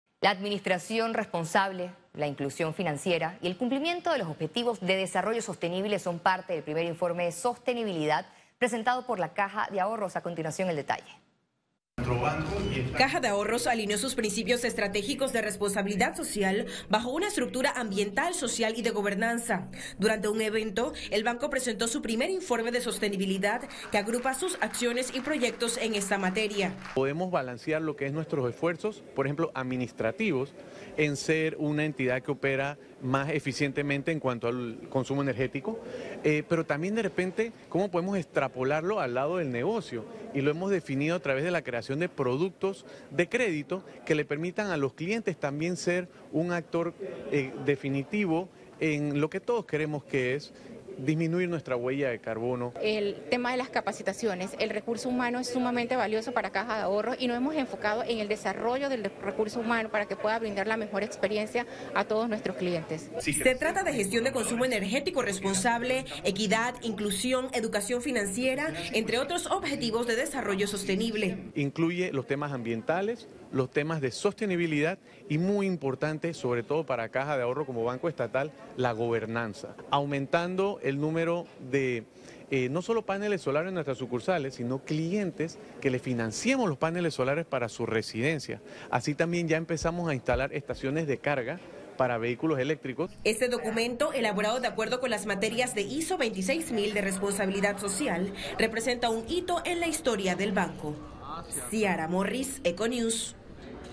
Caja de Ahorros presentó su primer informé de sostenibilidad
Panamá, 14 de febrero de 2023

Información por: Ciara Morris

La administración responsable, la inclusión financiera y el cumplimiento de los objetivos de desarrollo sostenible son parte del primer informe de sostenibilidad presentado por Caja de Ahorros.

Caja de Ahorros alineó sus principios estratégicos de responsabilidad social bajo una estructura ambiental, social y de gobernanza. Durante un evento, el banco presentó su primer informe de Sostenibilidad que agrupa sus acciones y proyectos en esta materia.

Se trata de gestión de consumo energético responsable, equidad, inclusión, educación financiera, entre otros objetivos de desarrollo sostenible.

Este documento, elaborado de acuerdo con las materias de ISO 26000 de Responsabilidad Social, representa un hito en la historia del banco.

Eco Tv Panama

Canal 28/1028 exclusivo de Tigo Panamá con Programas de Información especializada y de actualidad